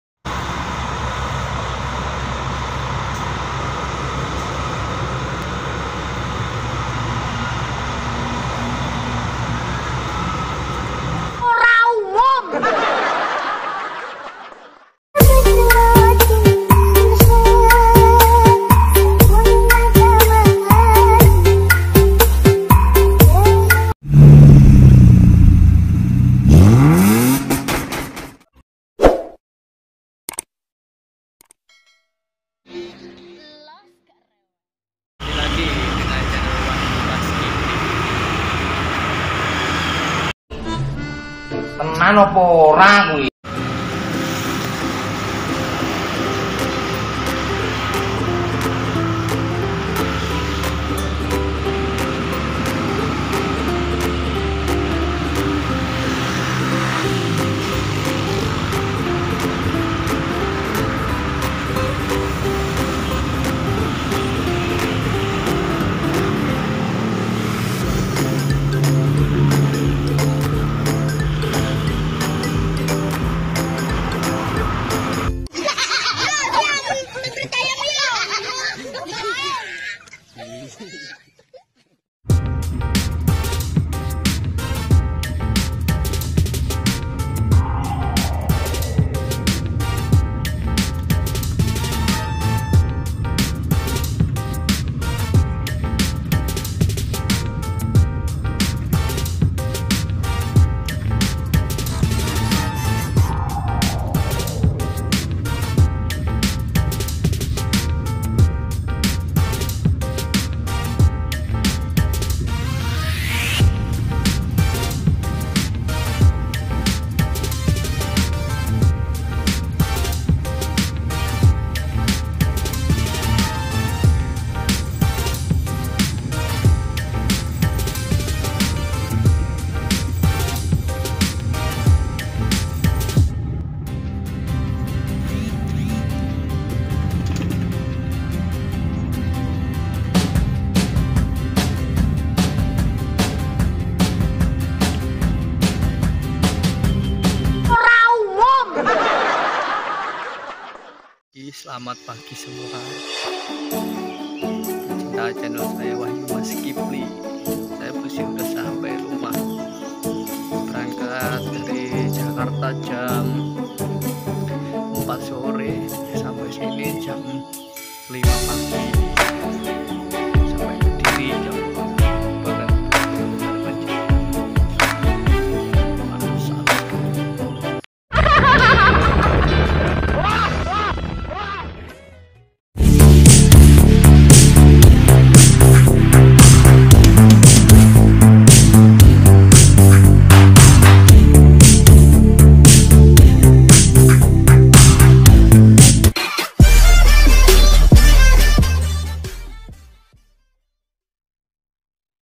NEKAT..!! Dari jakarta nekat Pulang ke rumah KEDIRI,demi BUKU NIKAH.
kali ini saya nekat PULANG KAMPUNG bareng bus malam PO.HARAPAN JAYA , naik dari klari,karawang timur.. ke rumah kediri.